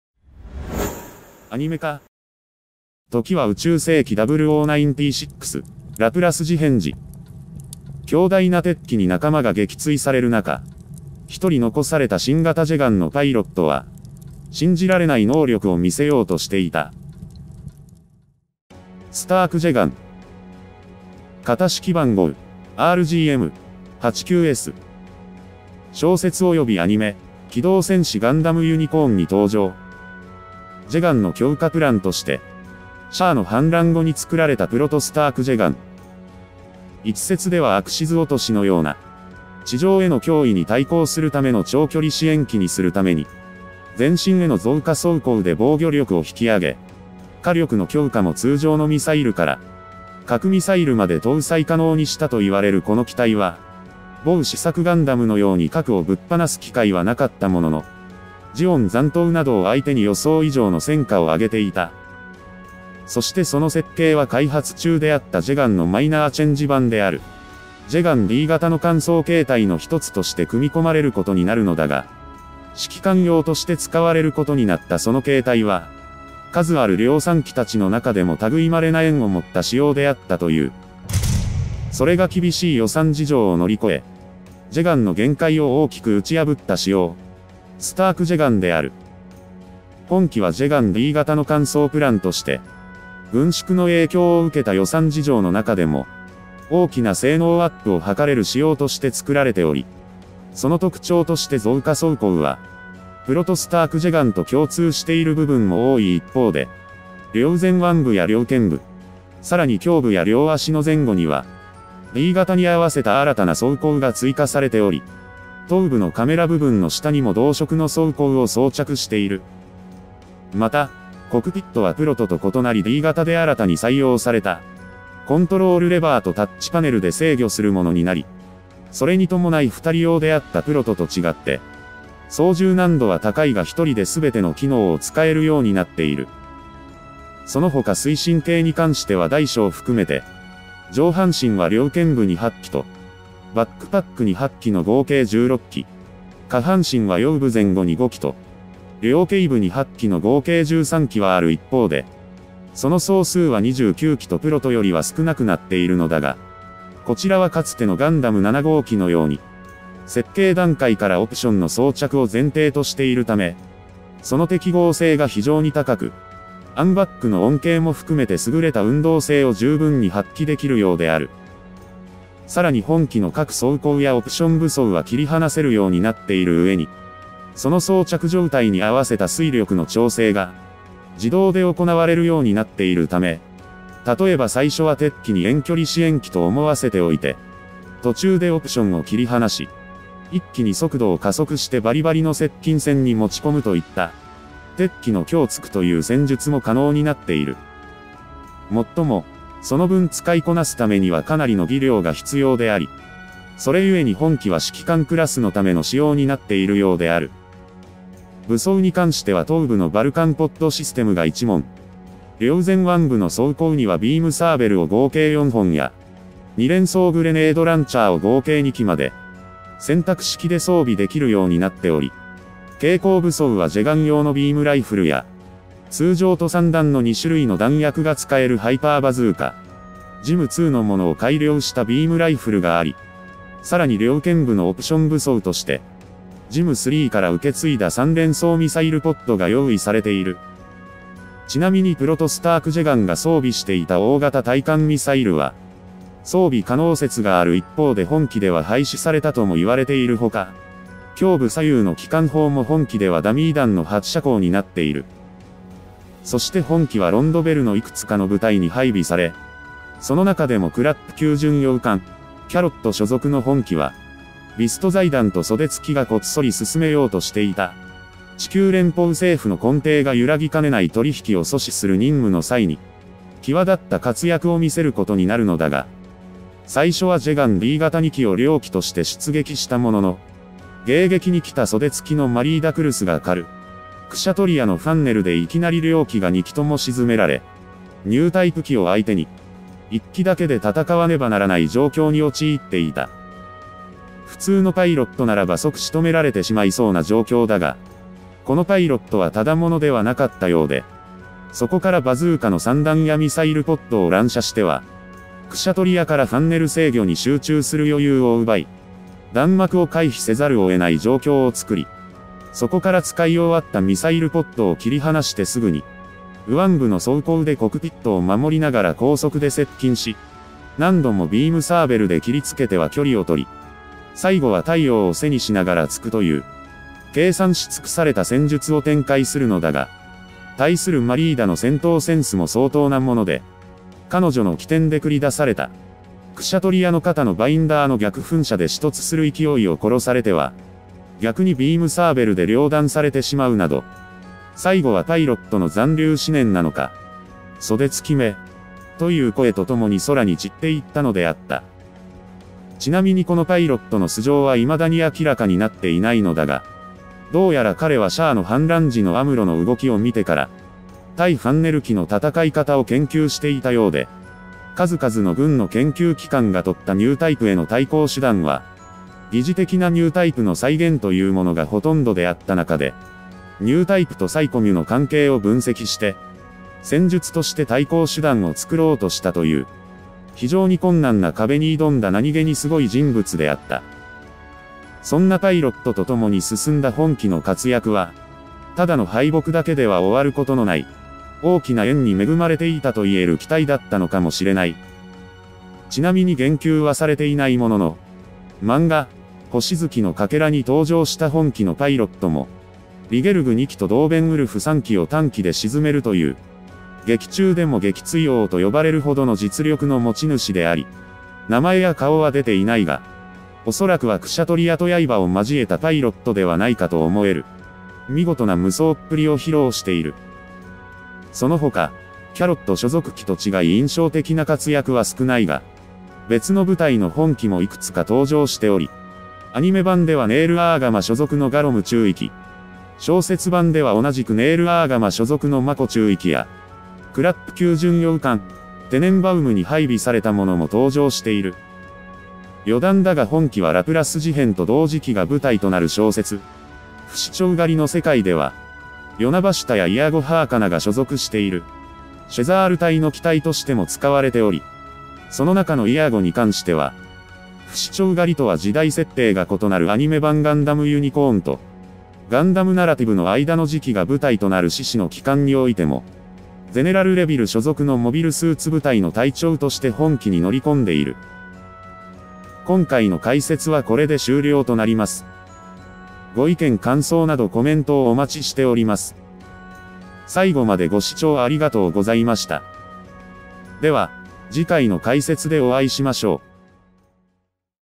脅威的過ぎる強さと共に謎を残した機体 スタークジェガンとは【ガンダムUC解説】
お待たせしました。動画投稿完了致しました！

今回はガンダムUCの序盤
当初の制作費をたった7分で使い切る原因と思われる
熱すぎる戦いを見せた機体であり
手に汗握る高度な戦いの中で
未だパイロットには謎が多く残されている機体
【スタークジェガン】になります。

素材：びたちー素材館
ナレーション：タカハシ（CEVIO）

各権利所有者・団体に帰属します。